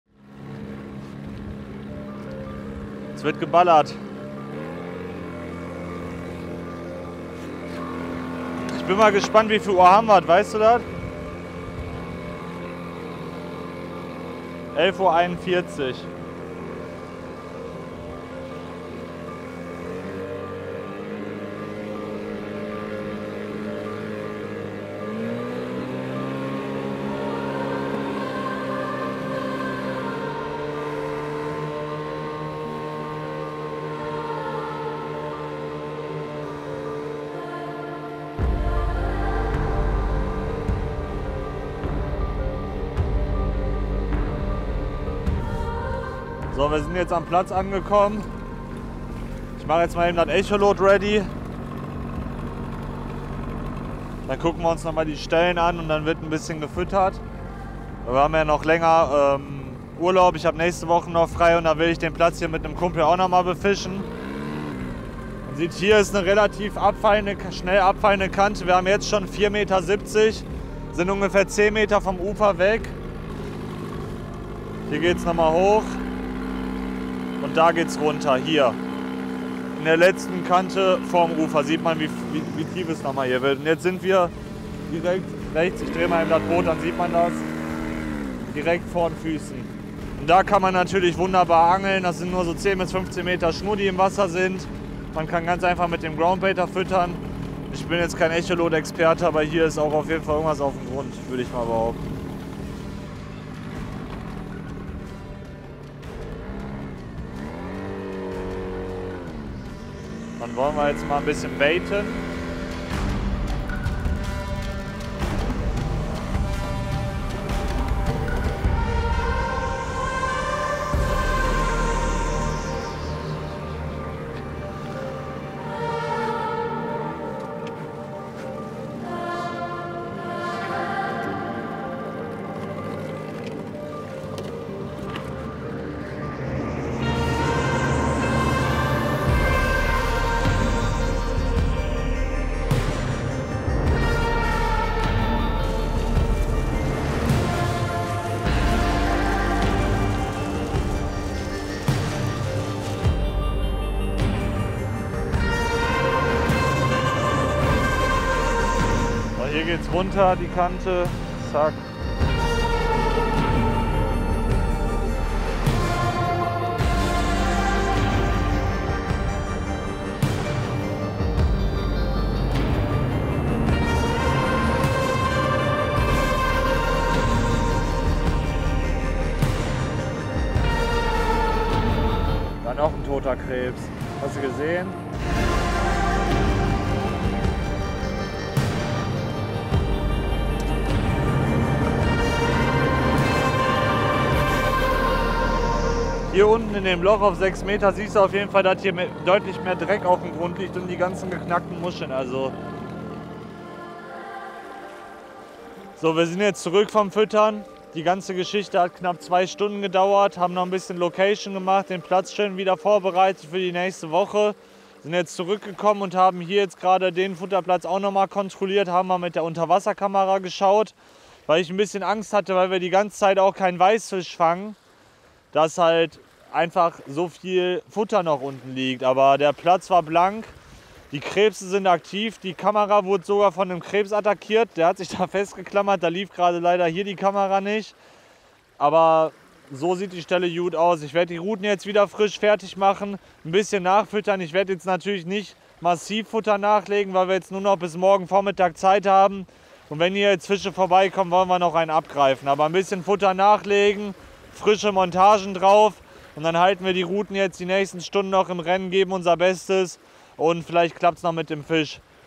Vater Rhein – die Königsdisziplin – Karpfenangeln am Fluss mit Zille & Flipper
Karpfenangeln am Fluss ist nicht jedermanns Sache und schon gar nicht an Deutschlands größtem Fluss, dem Rhein. Alex "Zille" Zilleckens und Philipp „Flipper“ Rüß haben sich ein Jahr dieser Herausforderung gestellt.

Wir begleiten die beiden NASH-Teamangler auf eine Session am großen Fluss und fragen sie rund um das Thema Rheinkarpfen aus. Spots, Futtertaktik, Anfüttern, Montagen, Hakenköder, Krabbenproblem und vieles mehr...

Den ganzen Film weiterschauen...